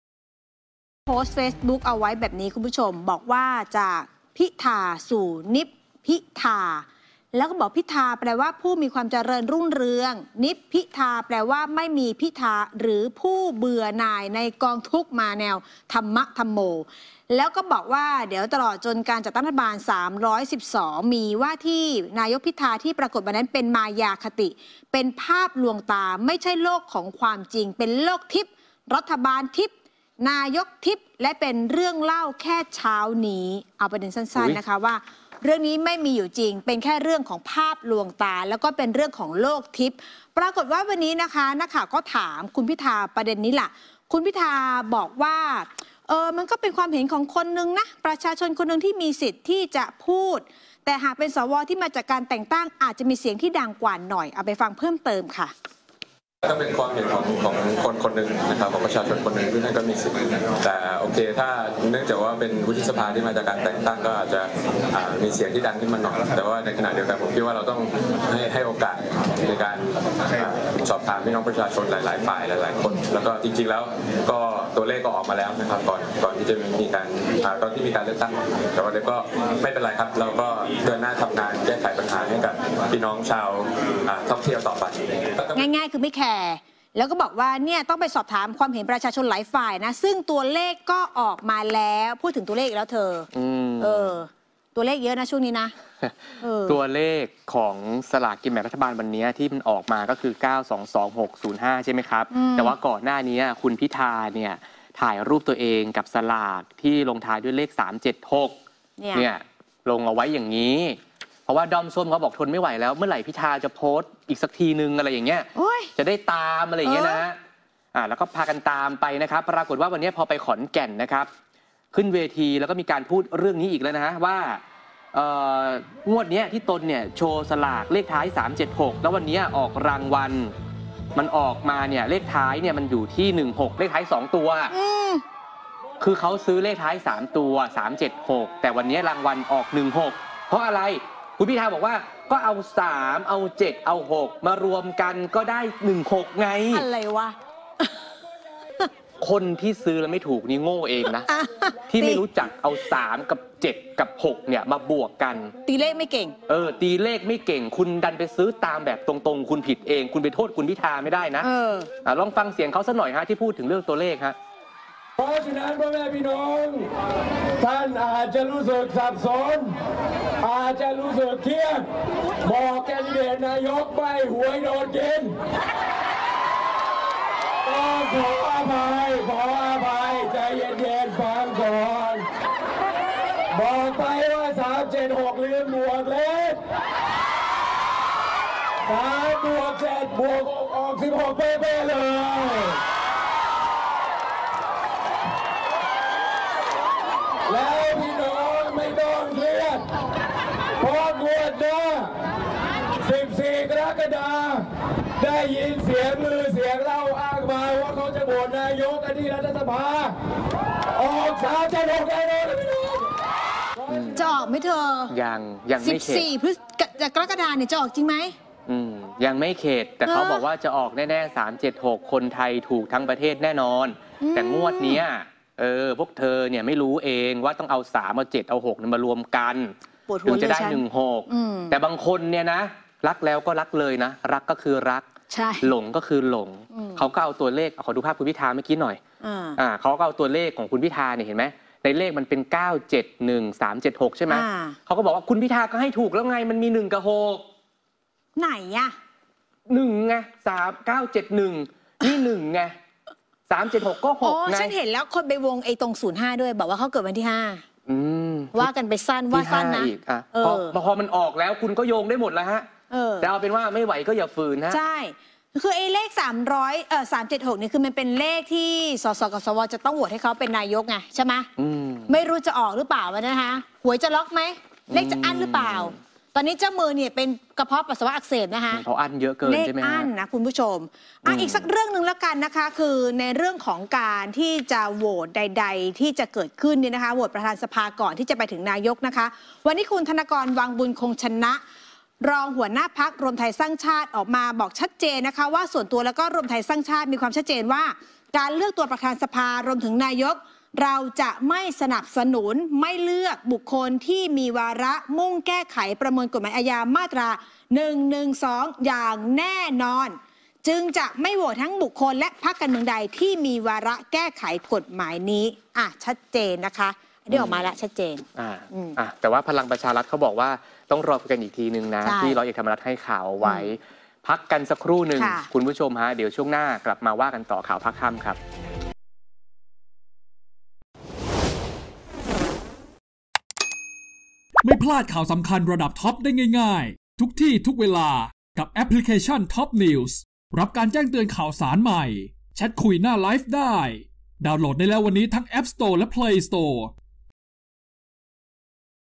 "พิธา" ขออภัยใบ้ตัวเลขไม่ตรงงวดนี้ มั่นใจ 14 ก.ค. โหวต "นายกคนที่ 30" ออก 376 แน่นอน | TOP HIGHLIGHT
"พิธา" ขออภัยใบ้ตัวเลขไม่ตรงงวดนี้ มั่นใจ 14 ก.ค. โหวต "นายกคนที่ 30" ออก 376 แน่นอน

ช่อง 77 : กล่อง PSI, INFOSAT, GMMZ, CSAT, TIK , 3BB, TOT
ช่อง 658 : กล่อง AIS
ช่อง 210 : IPM